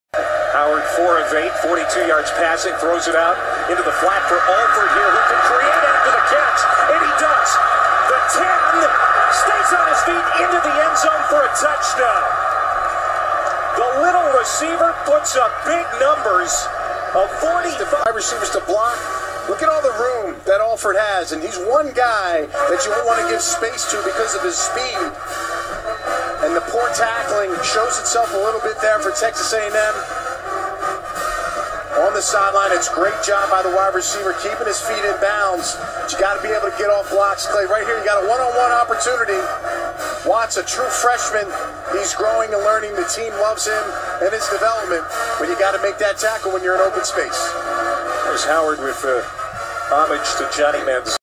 West Virginia University QB Skyler Howard make Money Sign like Johnny Manziel (Liberty Bowl 2014)
S.Howard complete to 5-M.Alford. 5-M.Alford runs 45 yards for a touchdown.